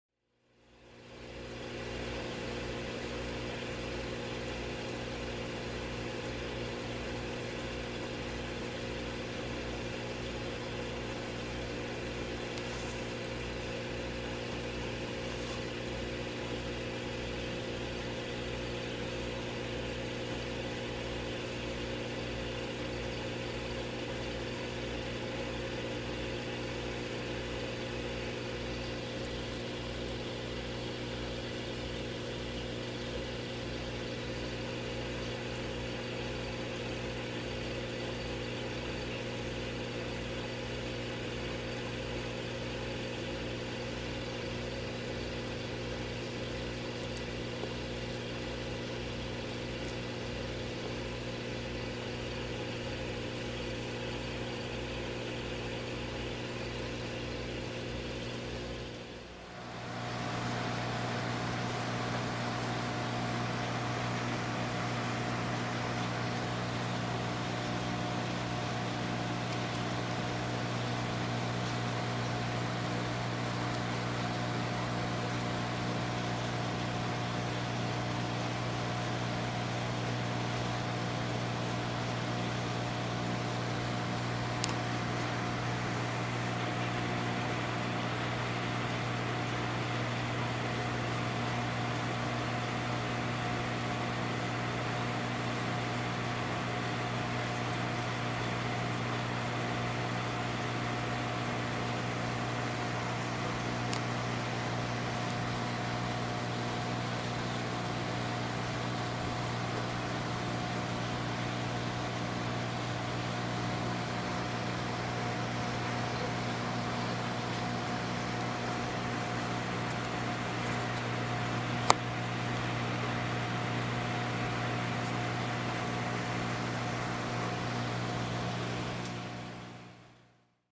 School of Clownfish in HD!
Clownfish Schooling. Something only seen in tank-reared clowns and never in the wild. Time for School!